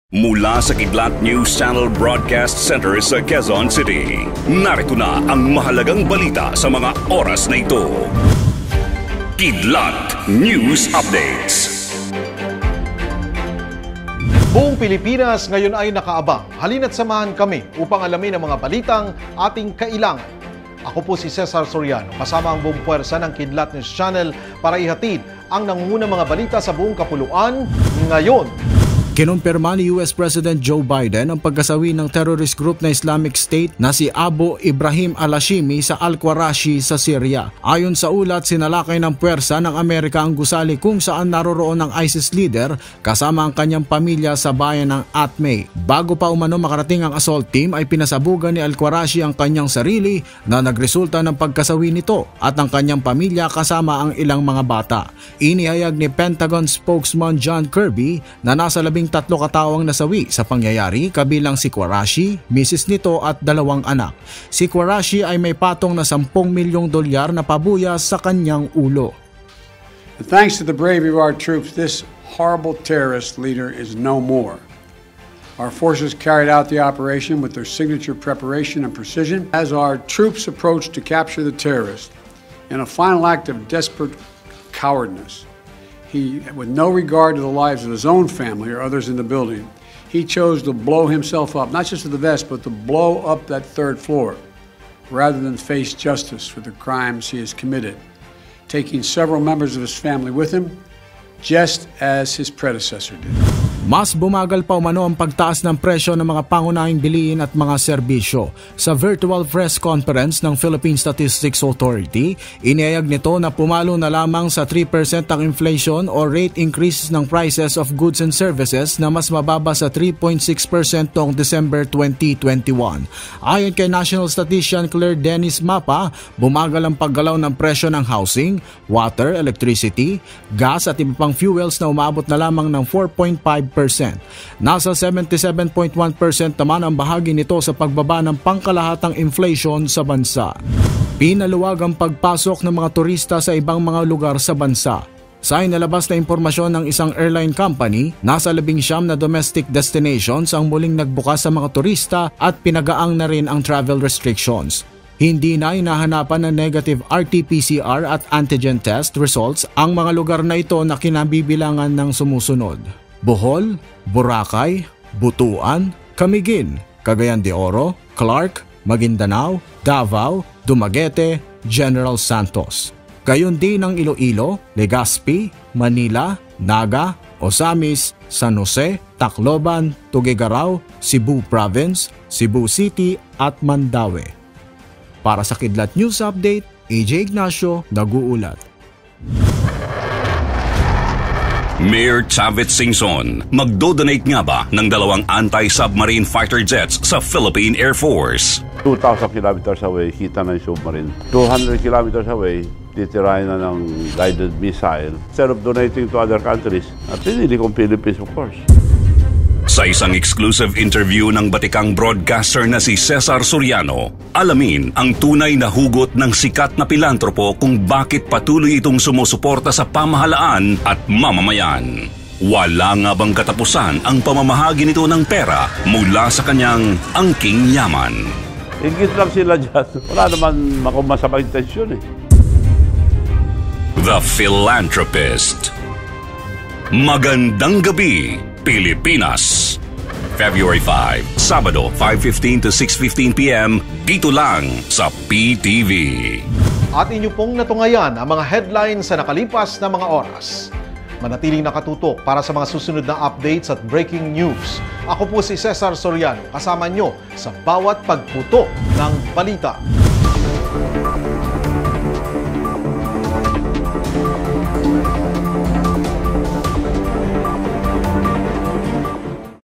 ISIS Leader Qurashi Patay Na | Kidlat News Update (February 04, 2022 2PM)
Kinumpirma ni US President Joe Biden ang pagkasawi ng terrorist group na Islamic State na si Abu Ibrahim Al-Hashimi Al-Qurashi sa Syria. Ayon sa ulat, sinalakay ng pwersa ng Amerika ang gusali kung saan naroroon ang ISIS leader kasama ang kanyang pamilya sa bayan ng Atme. Bago pa umano makarating ang assault team ay pinasabugan ni Qurashi ang kanyang sarili na nagresulta sa pagkasawi nito at kanyang pamilya kasama ang ilang mga bata. Inihayag ni Pentagon Spokesman John Kirby na nasa labing-tatlo katao ang nasawi sa pangyayari kabilang si Qurashi, misis nito at dalawang anak.  Si Qurashi ay may patong na $10-million na pabuya sa kanyang ulo.

Ngayon Na!